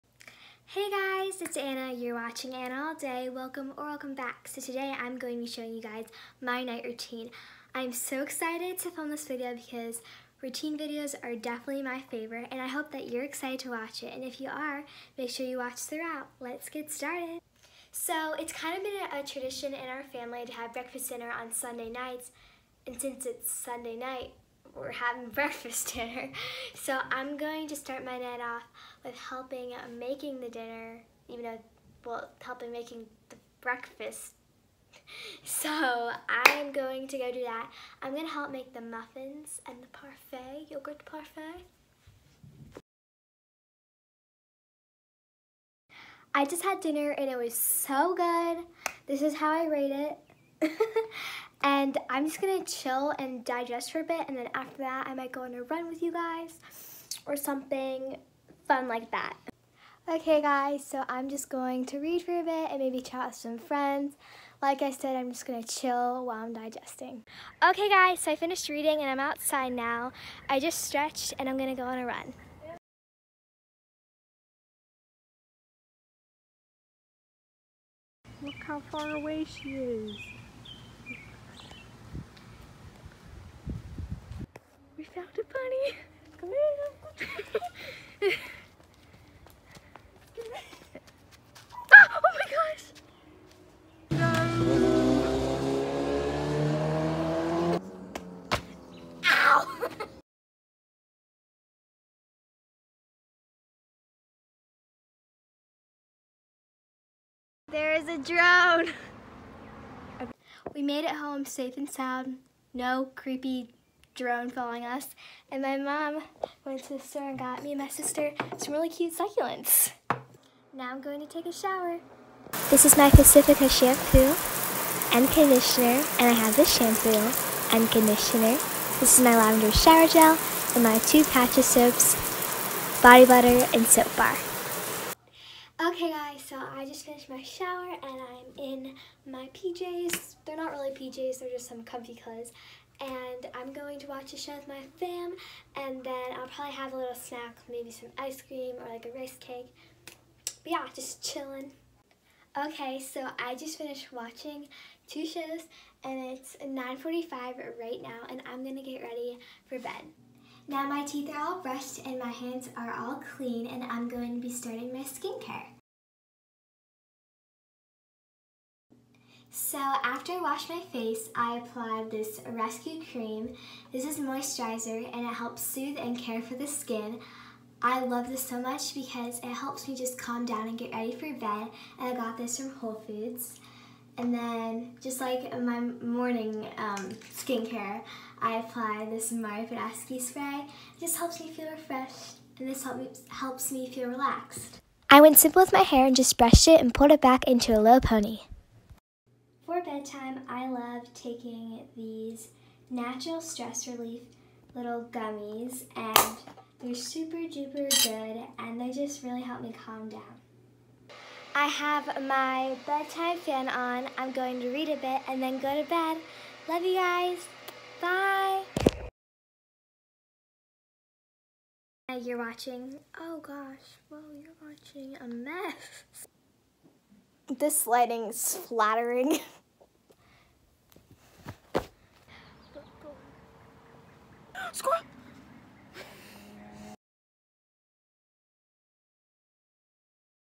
NIGHT ROUTINE// GET UNREADY WITH ME FOR BED// ANNA
night routine AnnaAllDay Hi! I hope you like my quarantine night time routine!